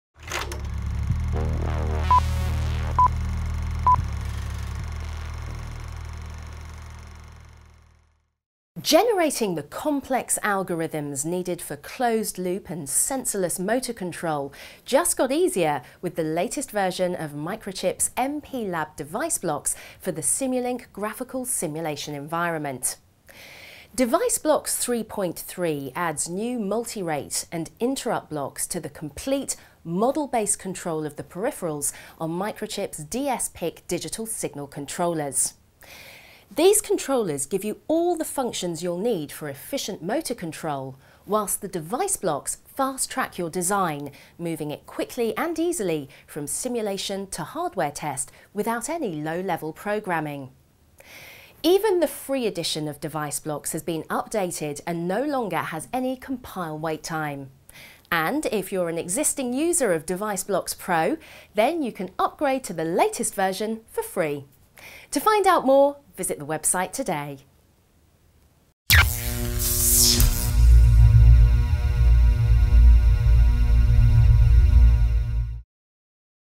New Device Blocks simplify complex motor-control algorithms
[MNV189] Microchip Technology Inc. introduces new Device Blocks that simplify complex motor-control algorithms